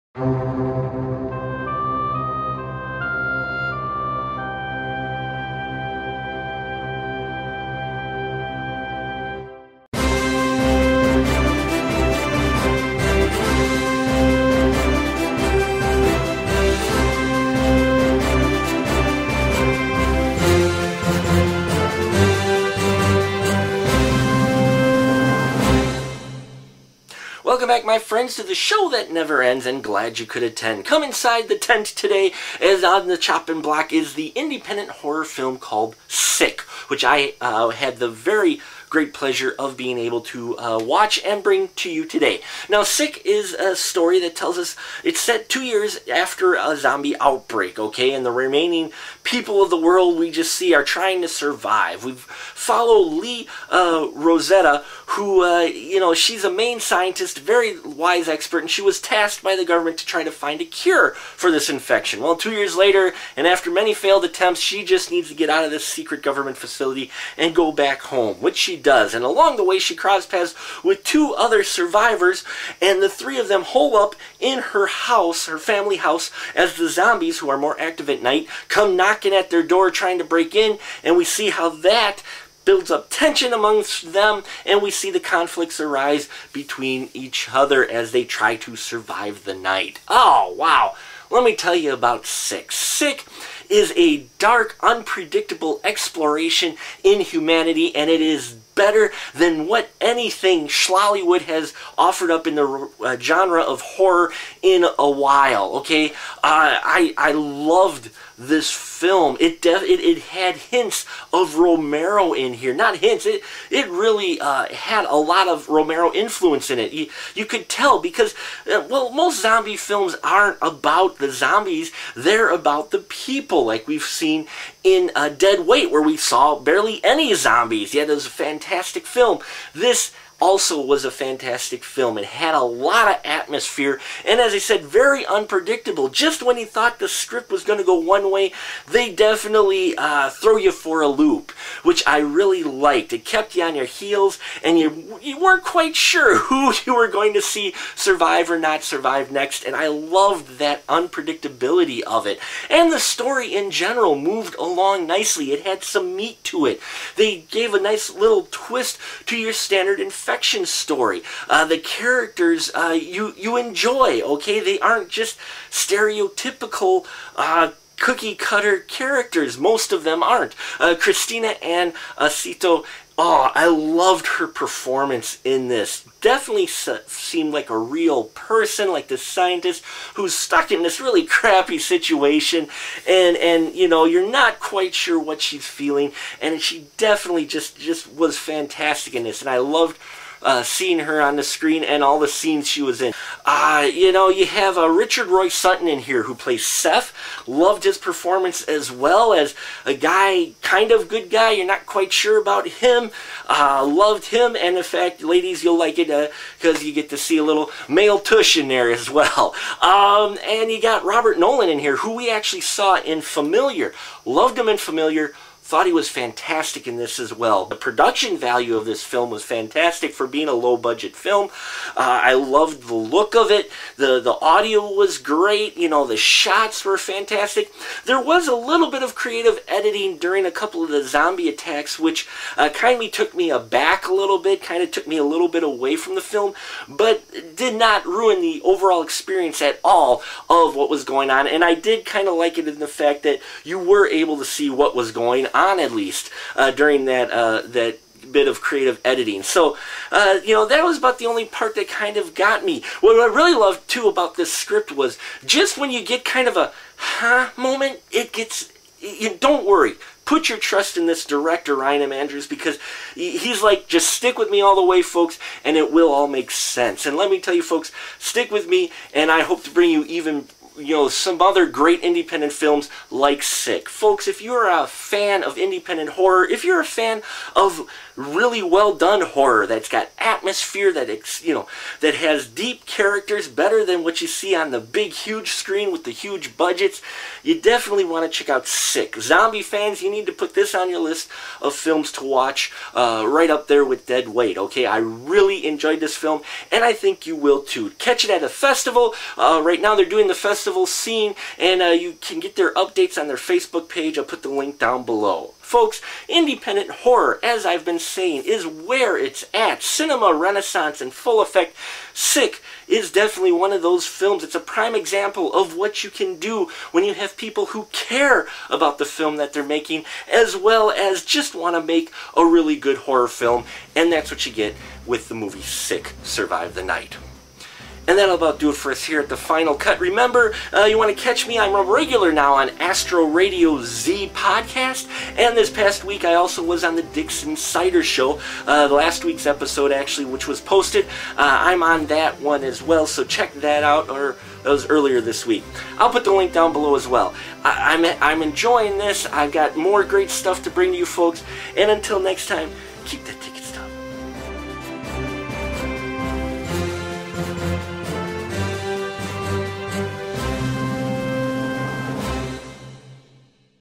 SICK "Survive the Night" (2012) Review on The Final Cut
We head back to the independent horror film department as we bring you our review of the film SICK. See if it survives the movieman.